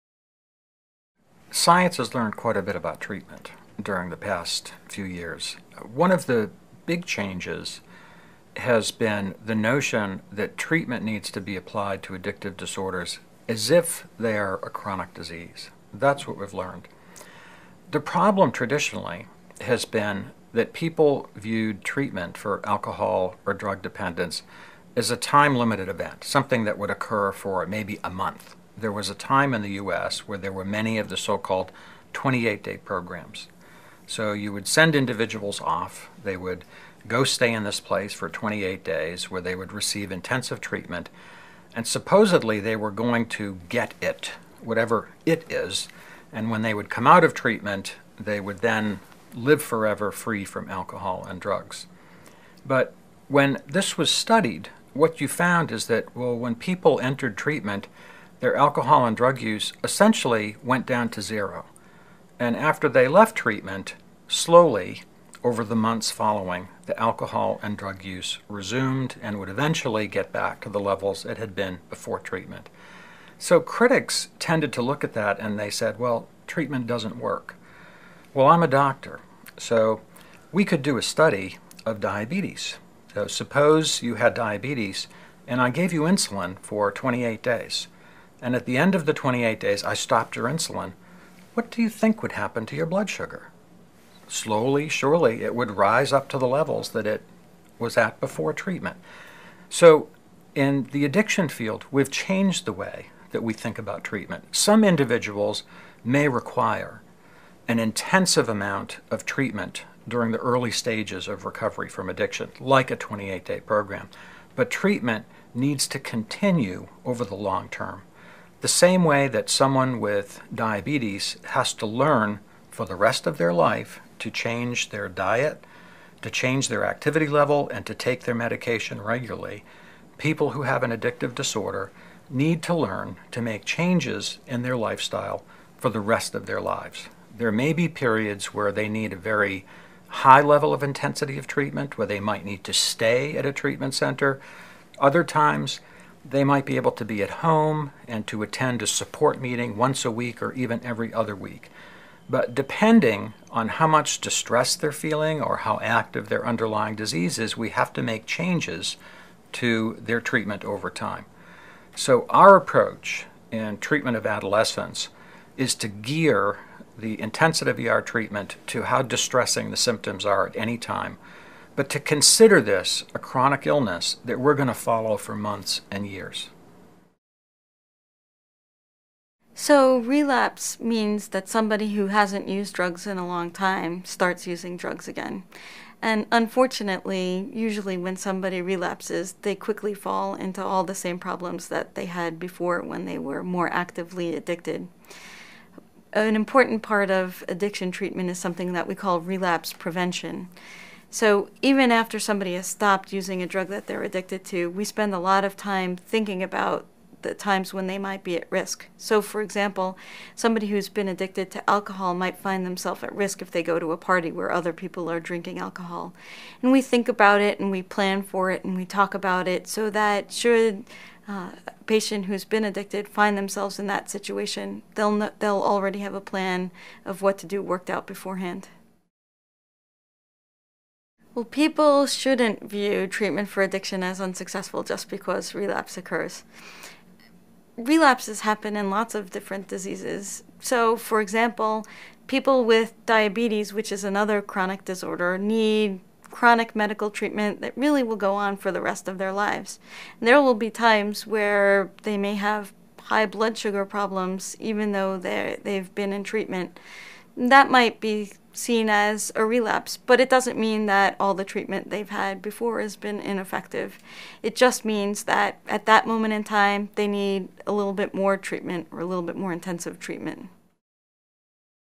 13. Is treatment for drug addiction effective?
A video excerpt from a BSCS module entitled Drug Abuse, Addiction, and the Adolescent Brain.